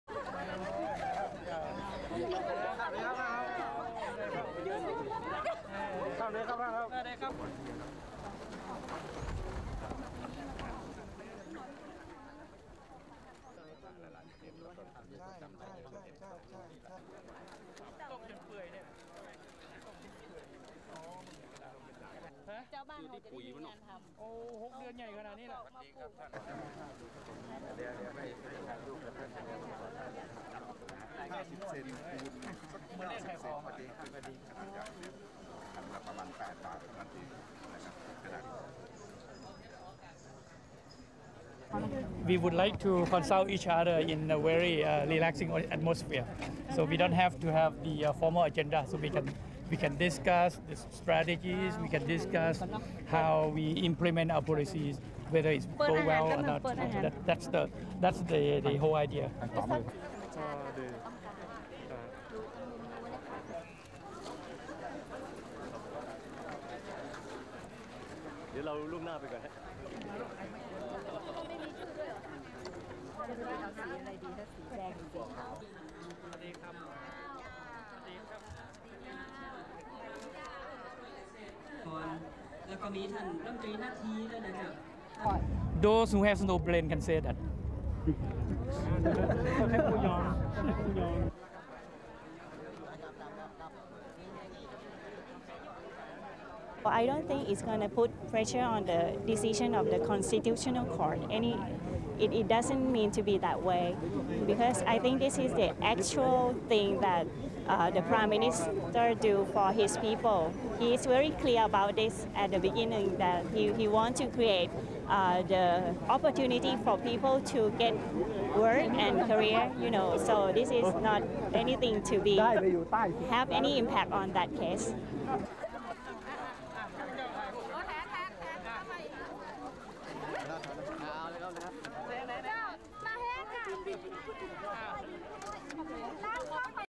PM's 3-day "mobile cabinet meeting" begins
1. Thaksin greets supporters
2. Thaksin walks through crowd
3. Photographers
4. Thaksin watches as woman stirs pot of boiling water, used in the production of "saa" paper
5. Thaksin
6. Crowd
7. Thaksin fixes decorations into tray of "saa" paper
8. Old women in market
9. SOUNDBITE: (English) Thaksin Shinawatra, Thai Prime Minister
"We would like to consult each other in a very relaxing atmosphere, so we don't have to have the formal agenda so we can discuss the strategies, we can discuss how we implement our policies and whether it's going well, and that's the whole idea."
10. Pongpol Adireksarn, deputy Prime Minister
11. Purachai Piemsomboon, Interior Minister
12. Sudarat Keyuraphan, Public Health Minister
13. Thaksin greets supporters
14. Crowd
15. SOUNDBITE: (English) Thaksin Shinawatra, Thai Prime Minister
"Those who have no brain can say that."
16. Thaksin walks through crowd
17. SOUNDBITE: (English) Nahathai Tewphaingam, Government Deputy Spokesperson ++NOT VERBATIM++
"Well I don't think it's going to put pressure on the decision of the constitutional court.  It is not meant to be that way because actually this (cabinet tour) is something the Prime Minister is doing for his people.  He was very clear about this at the beginning, that he wants to create the opportunity for people to get work and careers, so this is not something which is meant to have any impact on that (court) case."
18. Thaksin poses with supporters under paper umbrella

STORYLINE:

Thai Prime Minister, Thaksin Shinawatra, began a three-day series of "mobile" cabinet meetings on Monday, taking his ministers to the northern city of Chiang Mai.

But he's denied accusations the visits are designed to boost public support ahead of a crucial court ruling that could see him removed from office.

Thaksin continues to enjoy high levels of support among the Thai people and this visit to a craft village in his home province of Chiang Mai underlined that popularity.

A closely-packed throng of ministers, media and well-wishers crowded around as the Prime Minister was shown the process of making "saa" paper, a decorative wrapping made from mulberry leaves and an economic mainstay of the area.

The visit was aimed at showcasing and promoting the policies that got him elected last January - measures to boost the rural economy, provide low-interest loans and reduce poverty.

Similar field trips are expected to feature throughout the agenda of this three-day cabinet retreat, which the Prime Minister says is a chance to assess the progress made so far in achieving his ambitious goals.

The idea of taking ministers around the country in such a way is not new in Thailand but its timing is crucial.

The Constitutional Court will shortly rule whether Thaksin deliberately concealed the extent of his vast assets four years ago.

If he loses the case he'll be removed from office.

Some suggest this three-day visit is really about public relations - a move to pressure the judges by reminding them of his popularity.

Thaksin himself dismissed such criticism, with his spokesperson further insisting the tour stems from Thaksin's desire to reach out to his electorate.